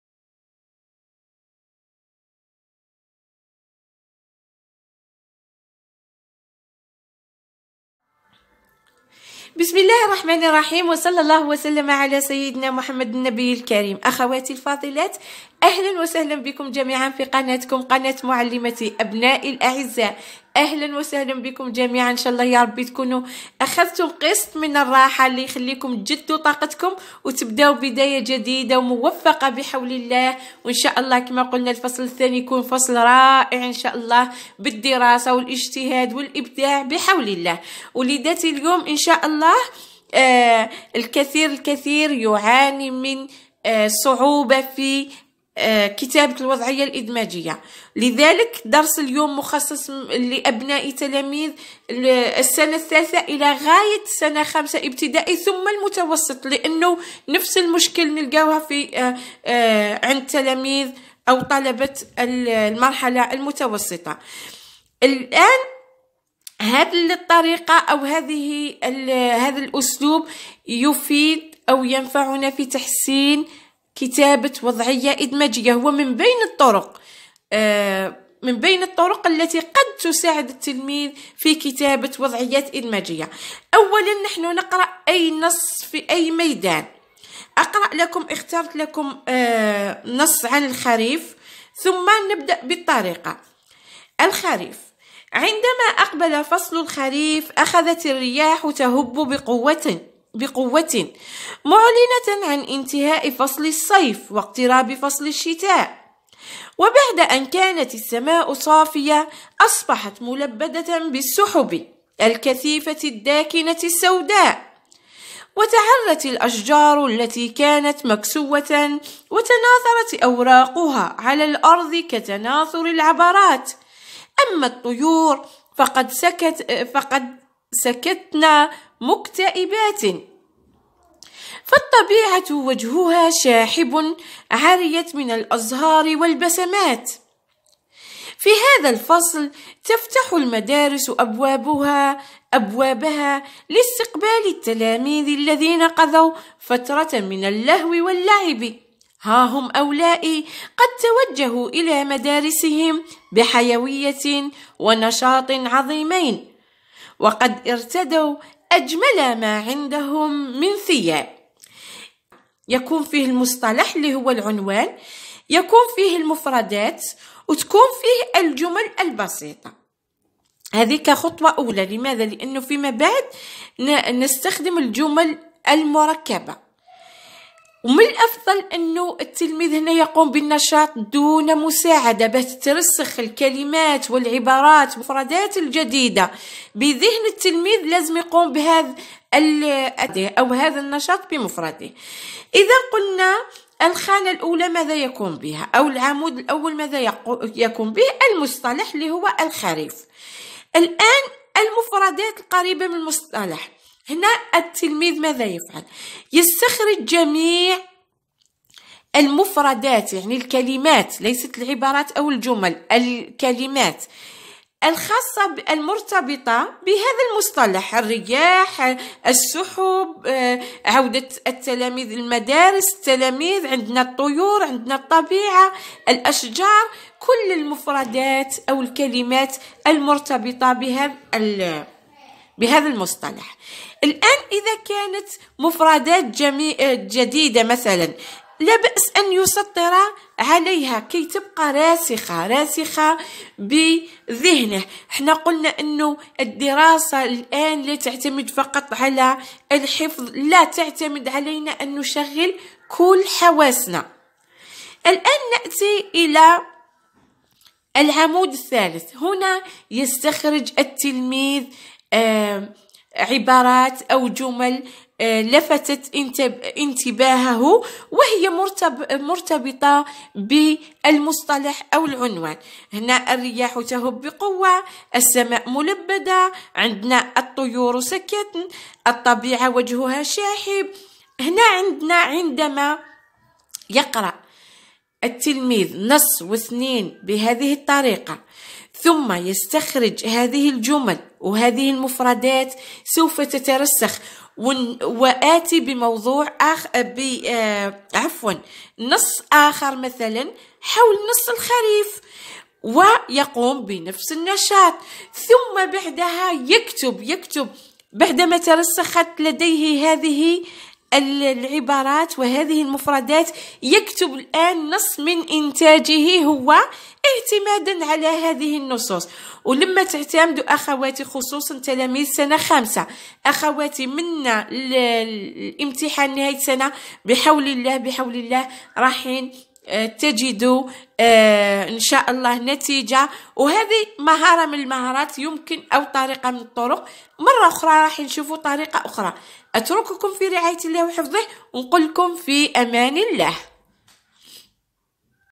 كيف أعلم أبني التعبير الكتابي تحسين قدرات التلميذ لكتابة وضعية ادماجية
مهارة أو طريقة نستعملها لتحسين قدرات التلميذ لكتابة نص حول موضوع معين